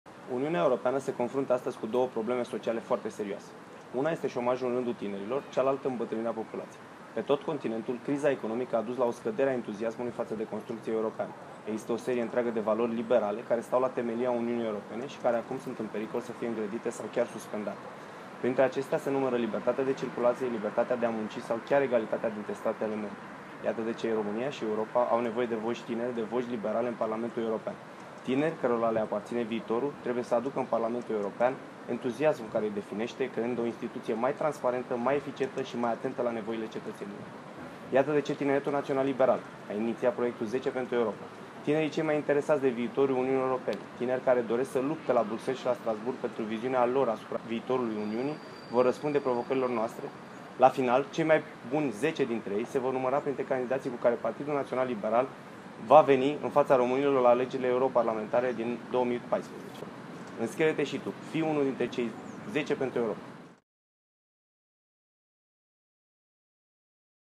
Florin Alexe si 10 pentru Europa
Uniunea Europeană se confruntă astăzi cu două probleme sociale foarte serioase. Una este şomajul în rândul tinerilor. Cealaltă, îmbătrânirea populaţiei.

Pe tot continentul, criza economică a dus la o scădere a entuziasmului faţă de construcţia europeană.
Există o serie întreagă de valorile liberale care stau la temelia Uniunii Europene, şi care acum sunt în pericol să fie îngrădite sau chiar suspendate. Printre acestea se numără libertatea de circulaţie, libertatea de a munci sau chiar egalitatea între statele membre.

Iată de ce România şi Europa au nevoie de voci tinere, de voci liberale în Parlamentul European. Tinerii, cărora le aparţine viitorul, trebuie să aducă în Parlamentul European entuziasmul care îi defineşte, creând o instituţie mai transparentă, mai eficientă şi mai atentă la nevoile cetăţenilor.

Iată de ce Tineretul Naţional Liberal a iniţiat proiectul „10 pentru Europa".

Tinerii cei mai interesaţi de viitorul UE, tinerii care doresc să lupte la Bruxelles şi Strasbourg pentru viziunea lor asupra viitorului Uniunii vor răspunde provocării noastre. La final, cei mai buni 10 dintre ei se vor număra printre candidaţii cu care Partidul Naţional Liberal va veni în faţa românilor la alegerile europarlamentare din anul 2014.

Înscrie-te şi tu! Fii unul dintre cei „10 pentru Europa"!